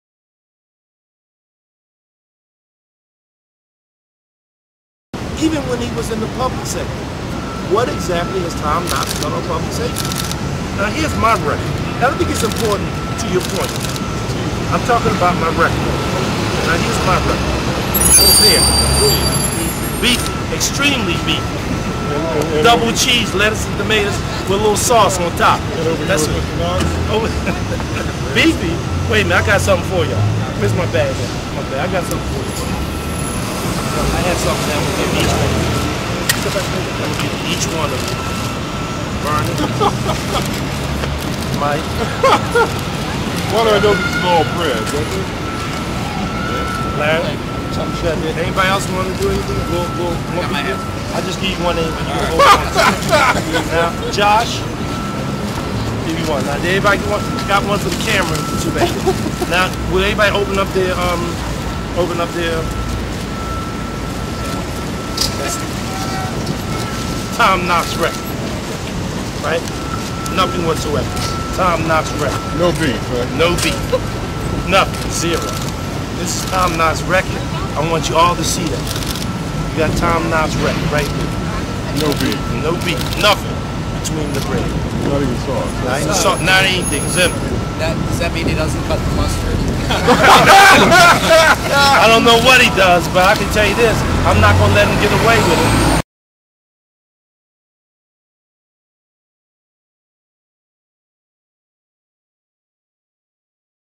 Dwight Evans to Tom Knox: Where's the Beef?
Video of Dwight Evans Press Conference comparing his "beefy" record of fighting crime to Tom Knox, who has no record of fighting crime.